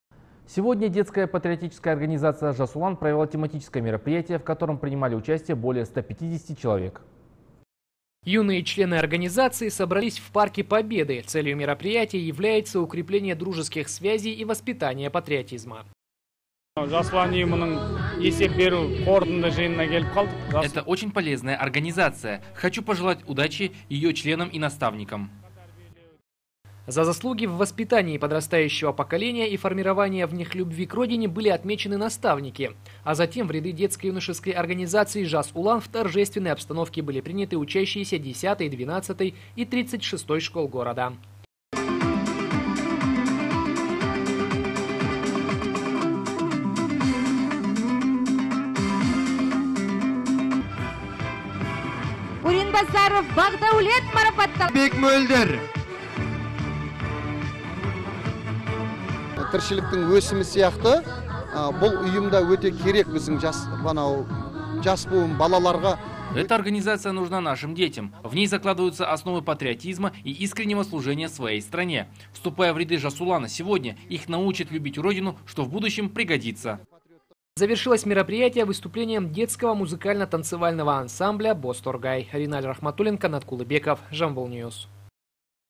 Ряды «Жас Улана» пополнились
Сегодня детская патриотическая организация «Жас Улан» провела тематическое мероприятие, в котором принимали участие более 150 человек.